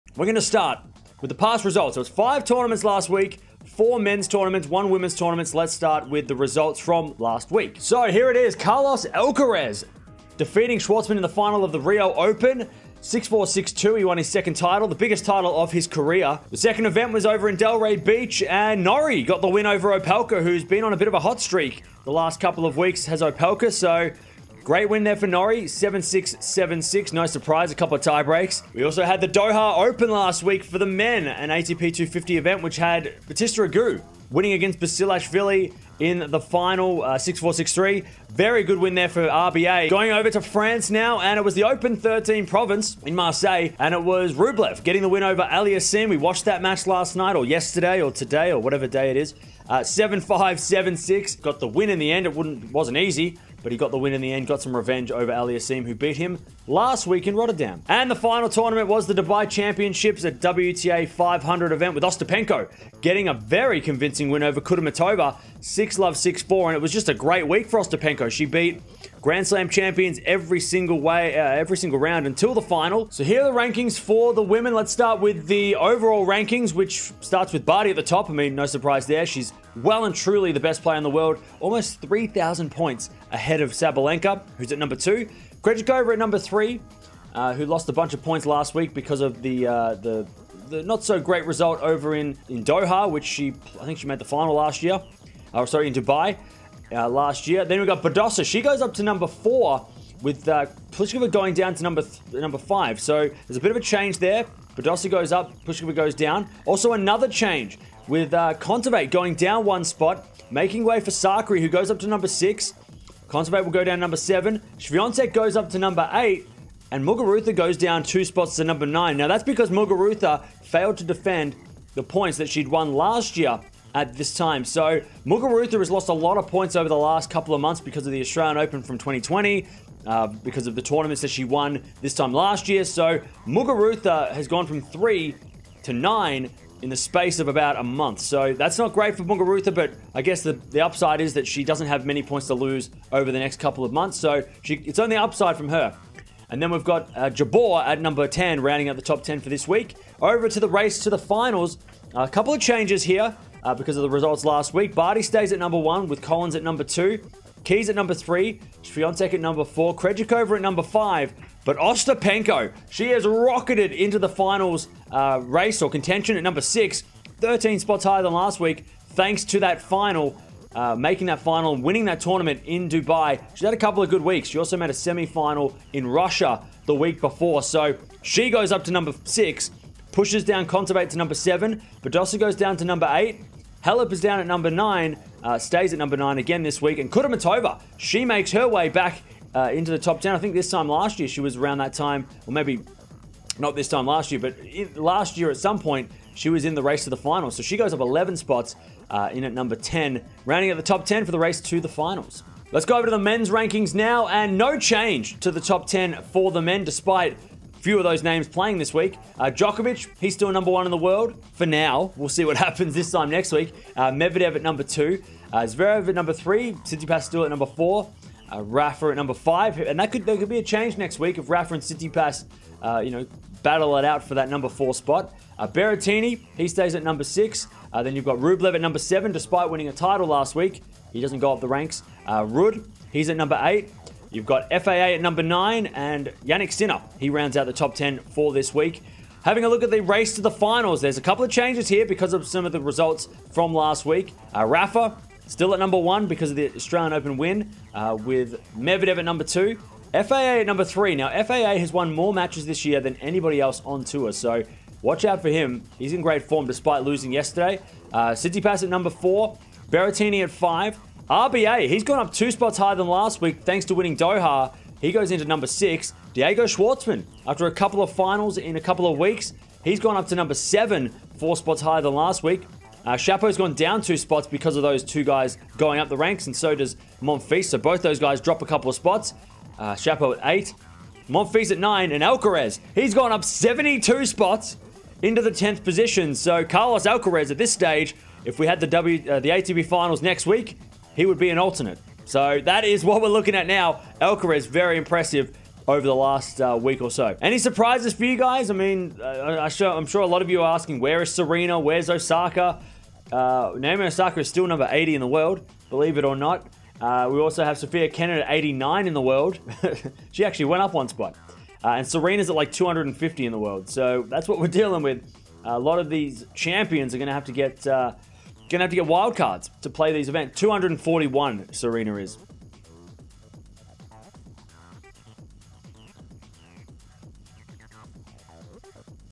Alcaraz, Ostapenko Win Titles | Muguruza Falls Again | Tennis Rankings News
After a jam packed week of the season and 5 tournaments happening around the world we had some epic moments. Carlos Alcaraz won his biggest trophy and Jelena Ostapenko continued her rise back to the Top 10 with a title in Doha

0:00 | Intro
0:07 | Past Results
1:13 | WTA Rankings
2:30 | WTA Finals Race
3:22 | ATP Rankings
4:00 | ATP Finals Race
4:57 | Outro